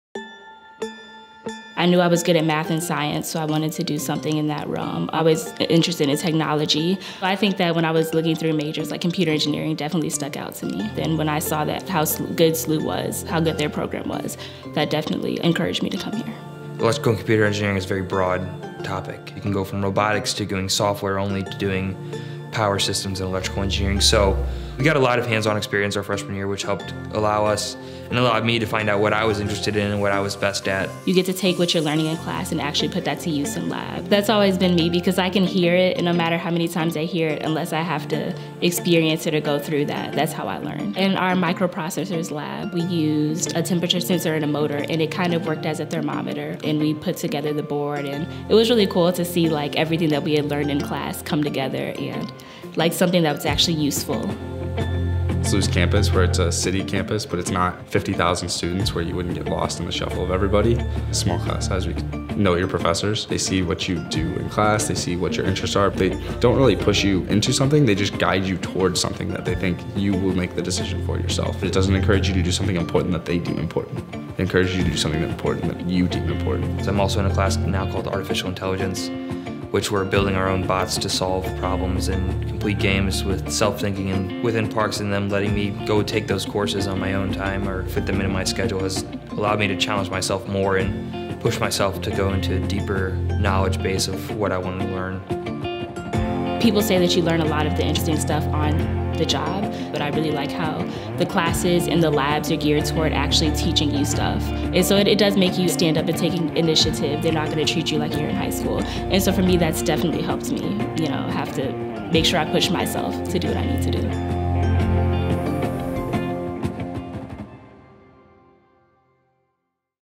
Why SLU Computer Engineering?
Ever wonder what sets Saint Louis University apart? Find out why our students chose SLU to pursue computer engineering.